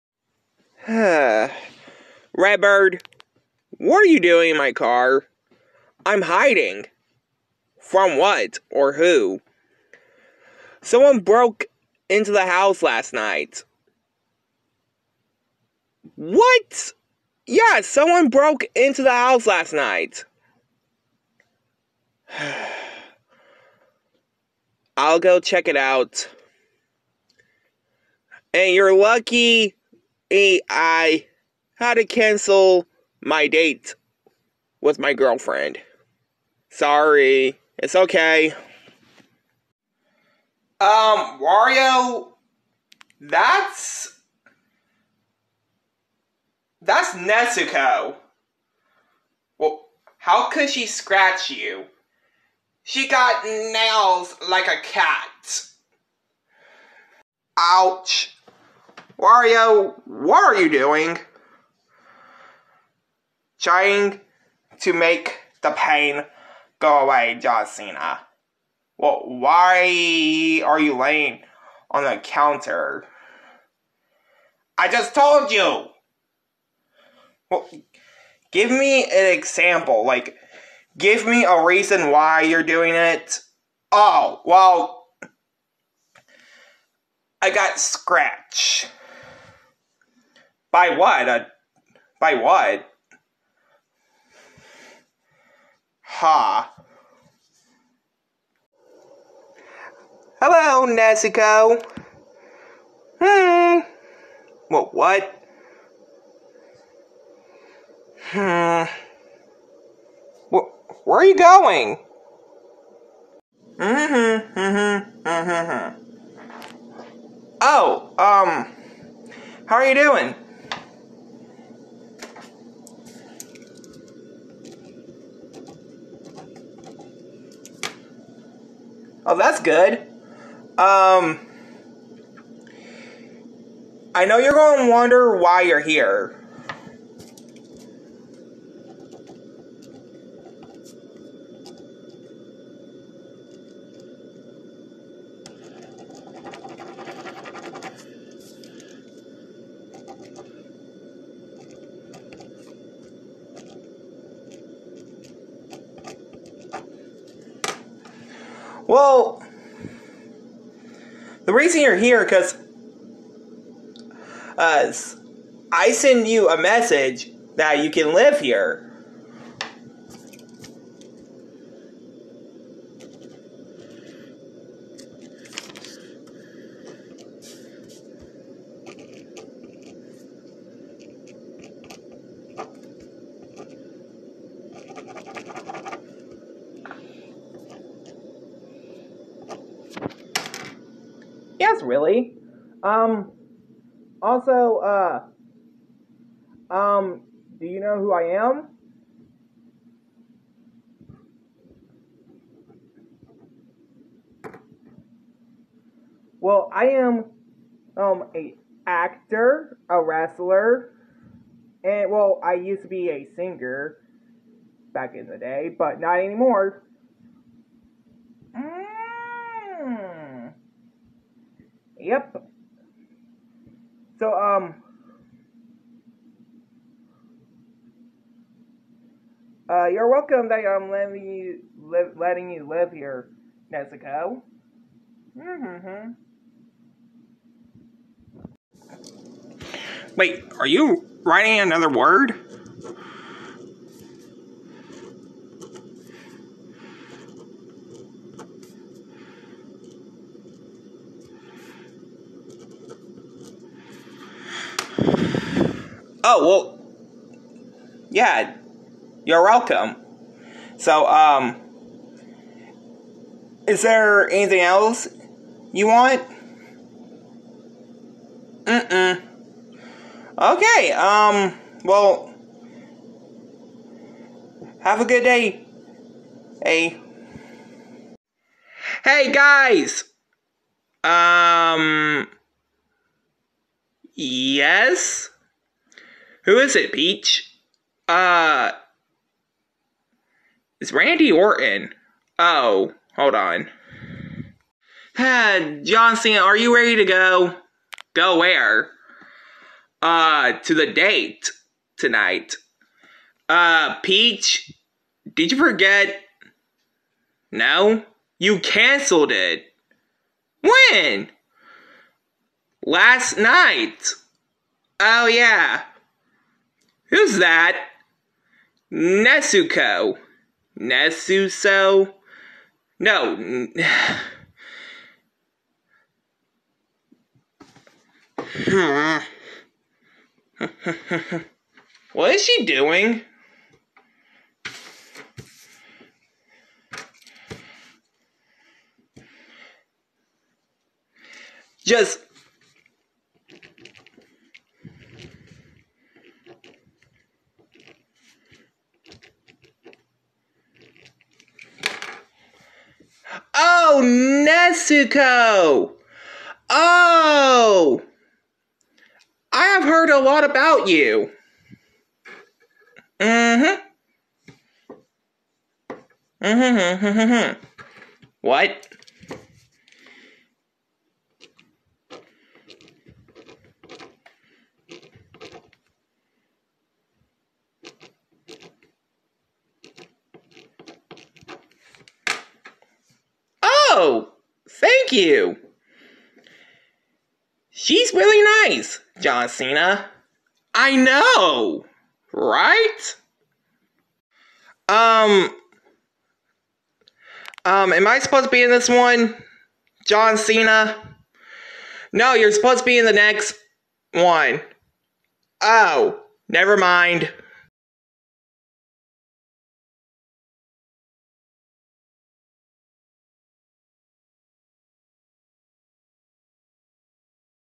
John Cena and The Angry Birds Season 8 Episode 4:Meet Nezuko!
In This Episode:
John Cena Finally Meets Nezuko
(New Plush Skit On February 16th)